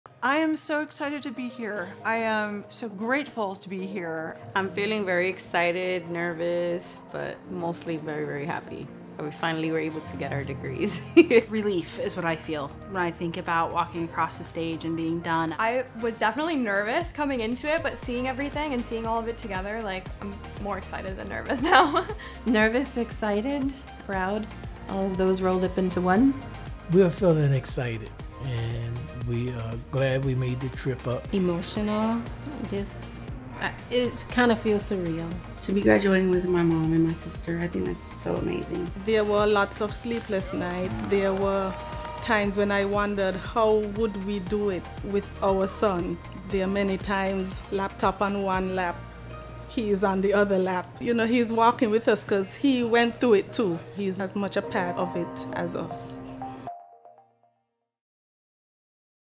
How it feels to be at SNHU Commencement
Hear from SNHU's newest alumni on how it feels to be at Fall 2022 Commencement.